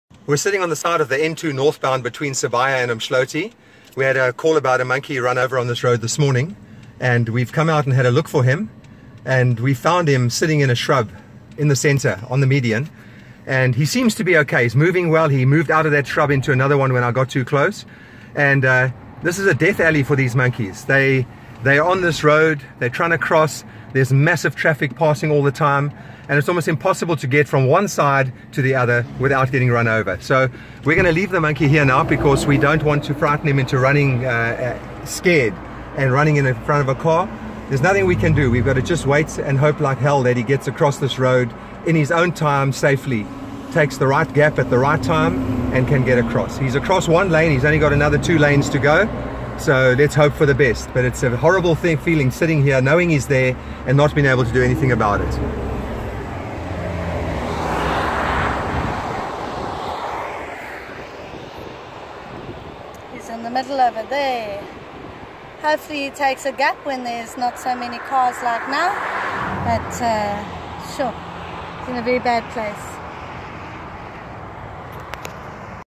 Road carnage.
Shhhhhoooo! HELPLESS!!
That's how we feel knowing that there's an adult Vervet monkey sitting in a shrub on the N2 median between Sibiya and Umhloti. The incident was reported to us by Ingrid Robertson who witnessed it en-route to work this morning. We travelled both sides of the freeway between Umhloti and Sibiya, about a three kilometre circuit which we did three times, even stopping in as safe an area as possible on the freeway so we could cross over to the grassy median and walk searching for him, expecting to find a mangled body or a severely injured male lying in the long grass. We eventually did find him, an alert male, seemingly uninjured and using all limbs, as we witnessed when he darted to the next shrub as we approached, after what may have been a concussion after his dice with death. With the adult male fully capable of making a quick dash we couldn't take the chance of trying to capture him, and in doing so forcing him to attempy yet another unplanned dash into the continuous row of speeding vehicles. So, in order for him to stand the very best chance of survival we had to leave him to wait until he felt fully capable of planning his dash at a time when he noticed a gap. Very stressful and worrying. All we can do, as we drive away, is hope and pray he completes his crossing safely. This is what dispersing adult males face every day as they move to new territories. Most don't survive. !